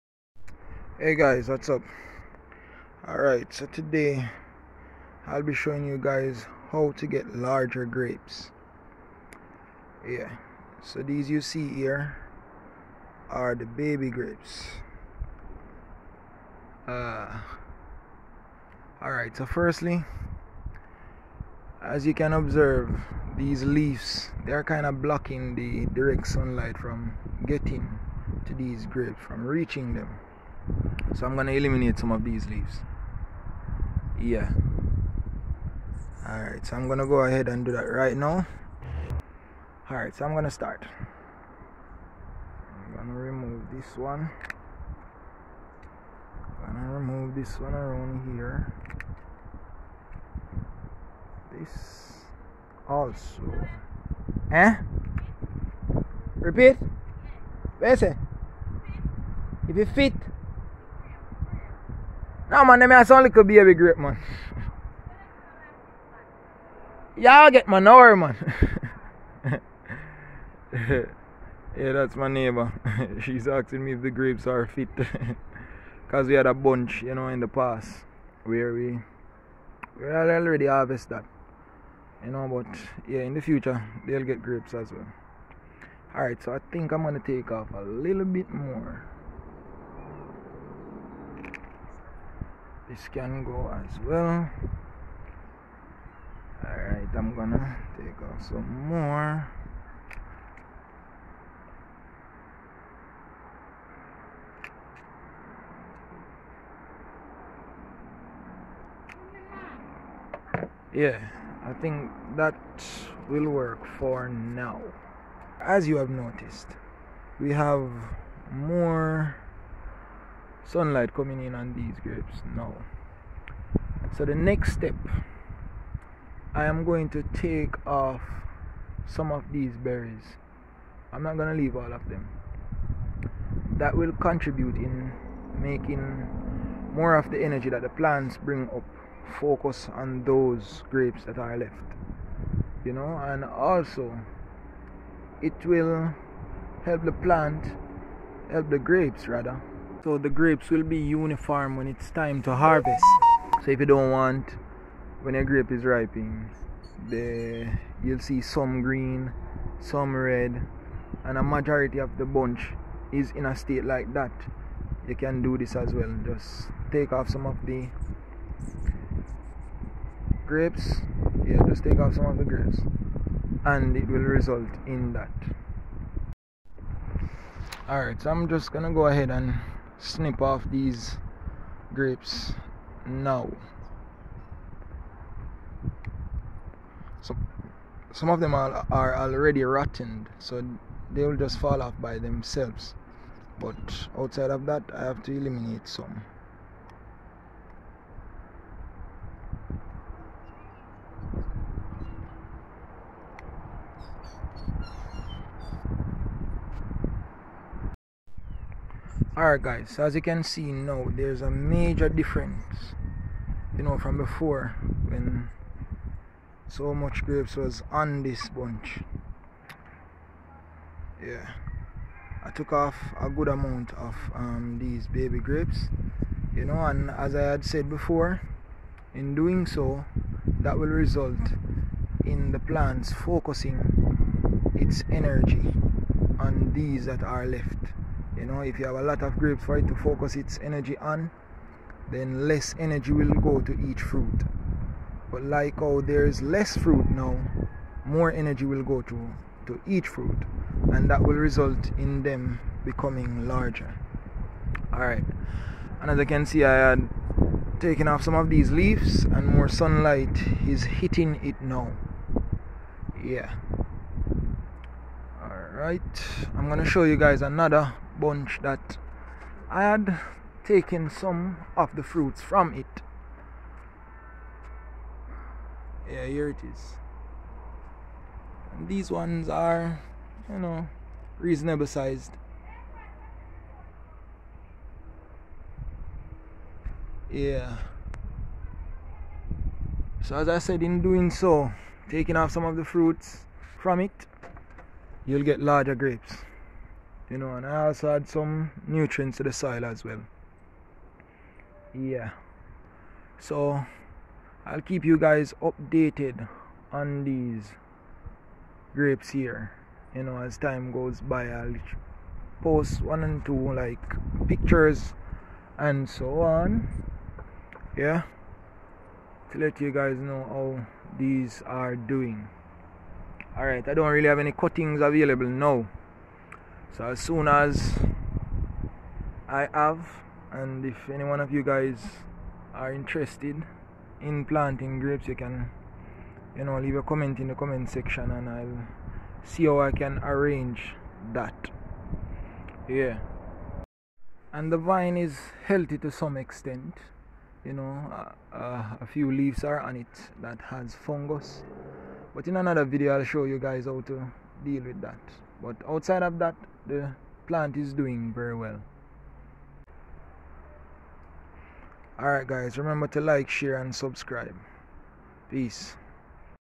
How To Grow Larger Grapes 🇯🇲
This video shows some strategies to use so you can grow larger grapes.

The grapevine in this video is the Red Cardinal Variety and it is grown in the parish of ST.CATHERINE in Jamaica.